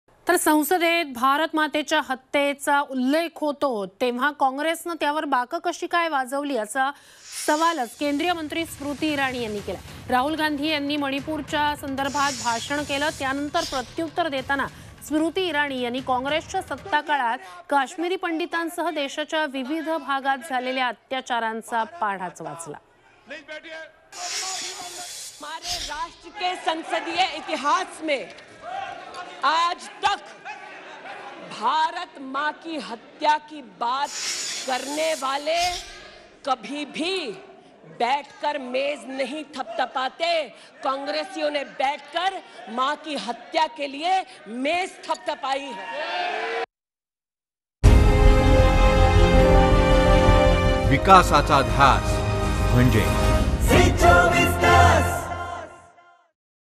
Rahul Gandhi यांच्या आरोपांवर Smriti Irani आक्रमक, काँग्रेस काळातील अत्याचारांचा वाचला पाढा
संसदेत भारतमातेच्या हल्ल्याचा होतो, तेव्हा काँग्रेसनं त्यावर बाकं कशी काय वाजवू शकतं, असा सवाल केंद्रीय मंत्री स्मृती इराणी यांनी केला. राहुल गांधी यांच्या मणिपूरवरच्या भाषणानंतर प्रत्युत्तर देताना स्मृती इराणींनी काँग्रेसच्या सत्ताकाळात काश्मीरी पंडितांसह देशाच्या विविध भागात झालेल्या अत्याचारांचा पाढा वाचला.

Union Minister Smriti Irani asked that when there is an attack on Mother India in the Parliament, how can the Congress play a role in it? Responding to Rahul Gandhi's speech on Manipur, Smriti Irani recounted the atrocities committed against Kashmiri Pandits in various parts of the country during the Congress rule.